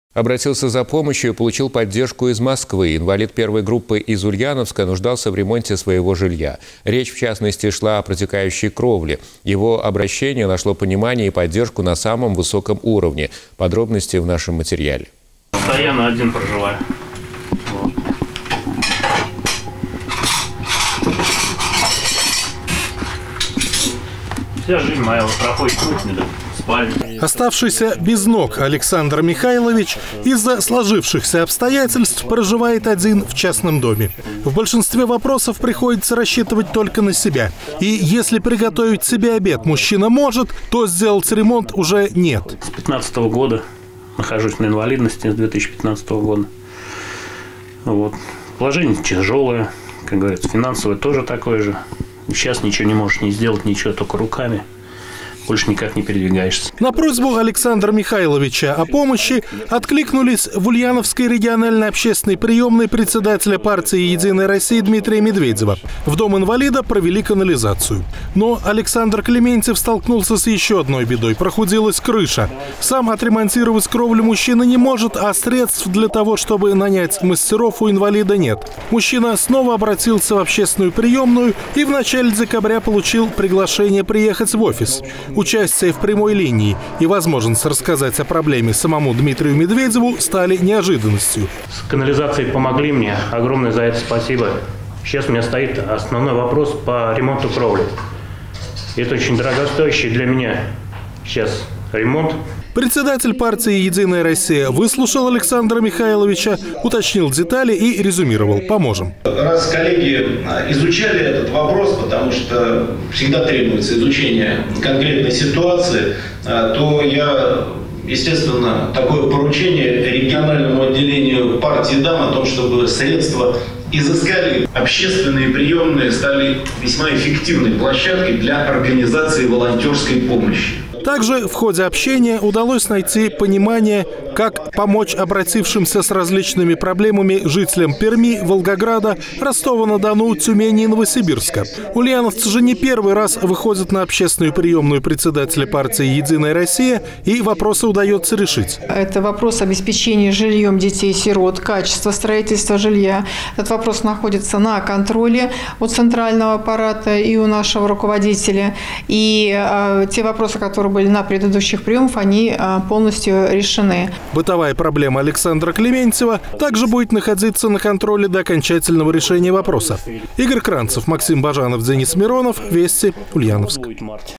Обратился за помощью и получил поддержку из Москвы
Инвалид первой группы из Ульяновска нуждался в ремонте своего жилья. Речь, в частности, шла о протекающей кровле. Его обращение нашло понимание и поддержку на самом высоком уровне.